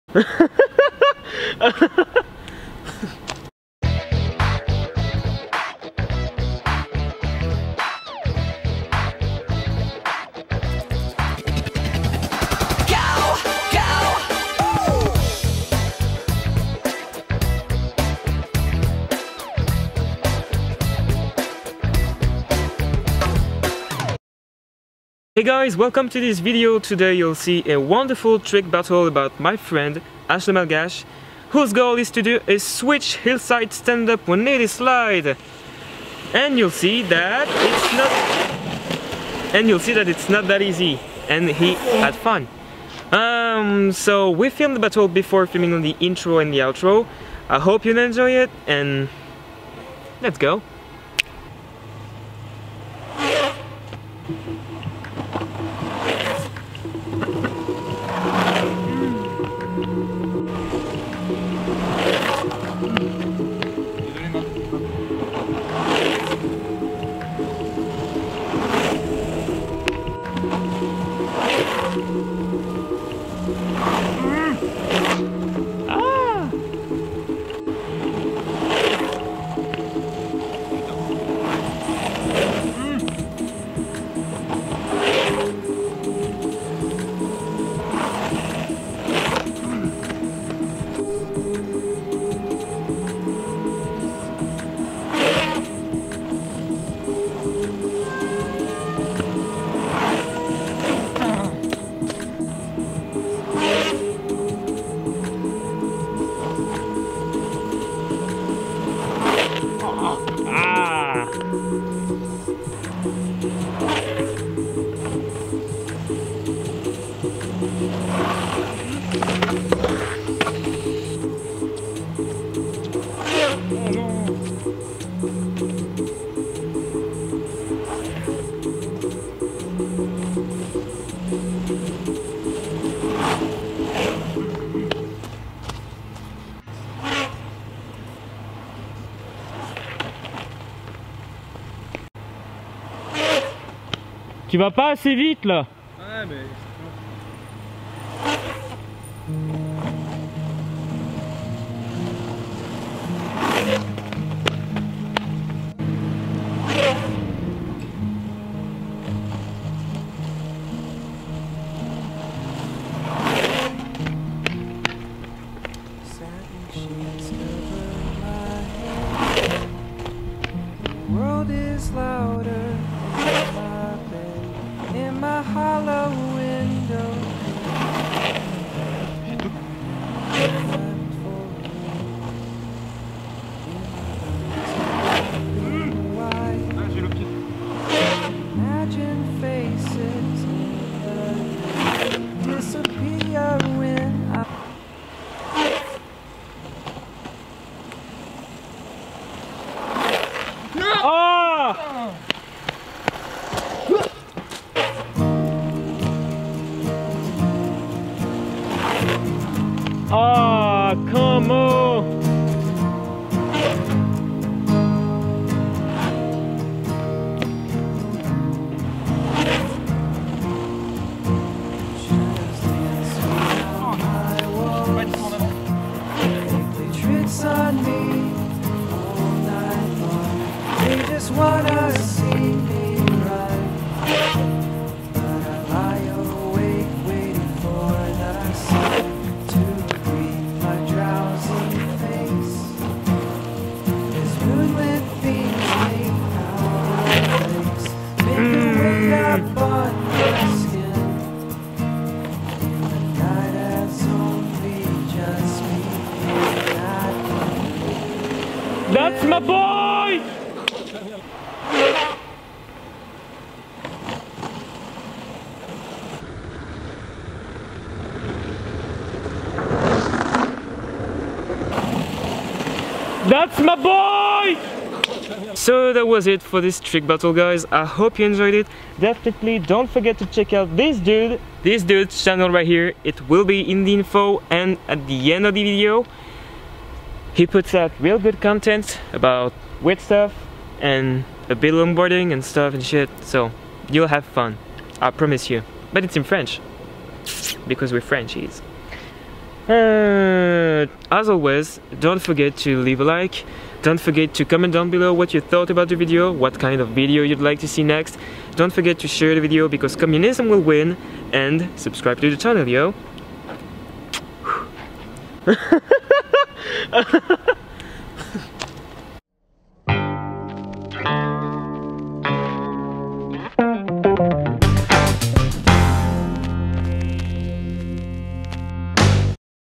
The Switch Heelside Stand-Up Slide w/ H le Malgash (Slide Battle) (sous-titré)
Hey my doods,

Hope you'll enjoy today's video :)

It's about a good friend o' mine learning how to ride switch :)

Here is a really easy Slide Battle on which H struggled

Featured people that helped me :

H le Malgash

This video features an Elixir

Check my IG page

Ride Safe

Byah!!